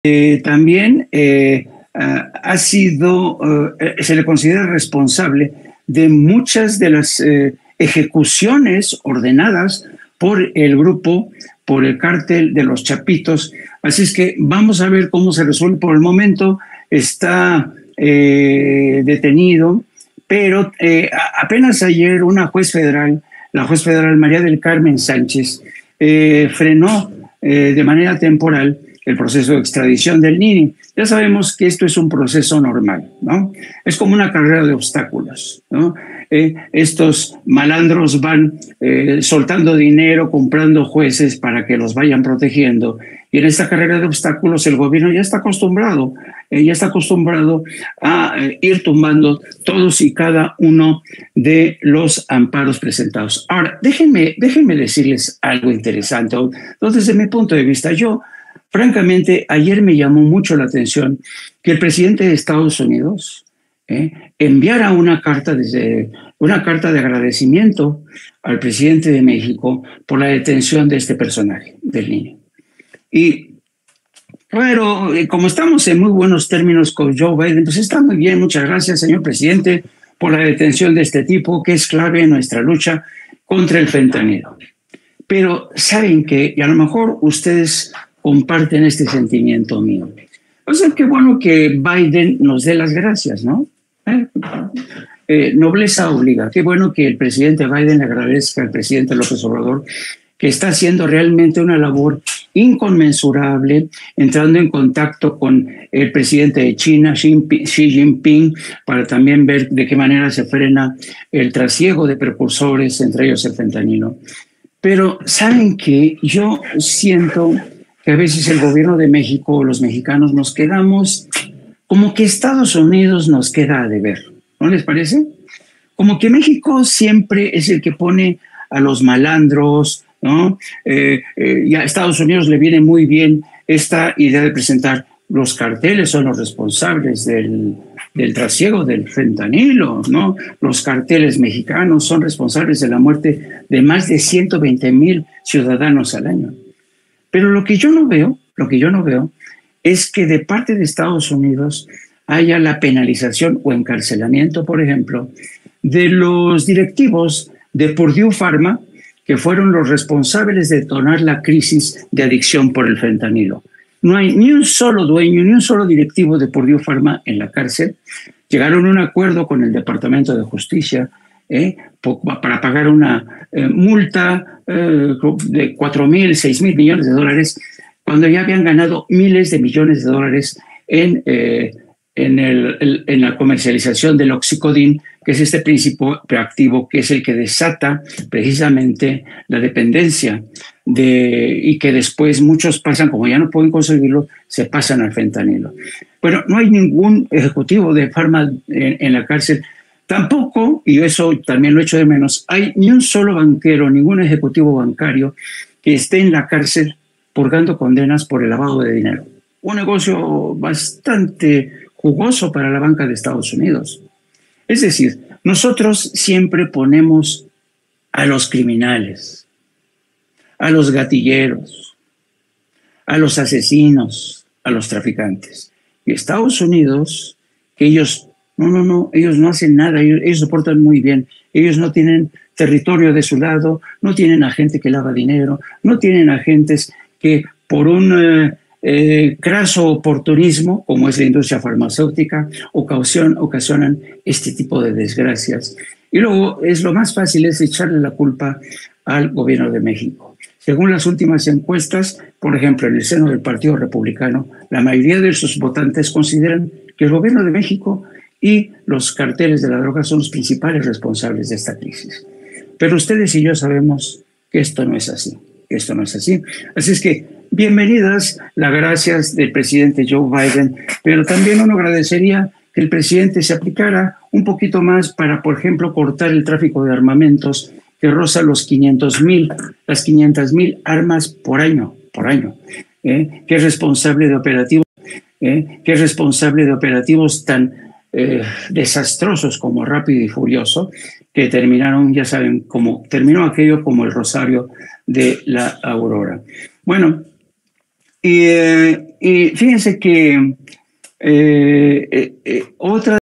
MUY BIEN EL AGRADECIMIENTO DE BIDEN, PERO ¿Y LA CHAMBA DE EEUU PARA COMBATIR EL TRÁFICO DE DR0G4S?
La detención de “El Nini” uno de los g0lp3s más eficaces a la d3l1ncu3nc14 y hoy, en la mañanera, el Gral. secretario de la Defensa Luis Cresencio Sandoval confirmó que fue él el orquestador del “culiacanazo”, entonces, ¿dónde quedó esa supuesta protección del presidente López Obrador al “Chapo” Guzmán? Y en este sentido, bien que el presidente Joe Biden agradezca el trabajo de México tras esta detención, pero ¿dónde está la chamba de su gobierno para detener por ejemplo a los responsables de detonar la dependencia hacia las dr0g4s o los banqueros por lavado de dinero? Ya estuvo bueno de que México siempre ponga a los cr1m1n4l3s, a lo 4s3s1n0s, a los tr4f1c4n.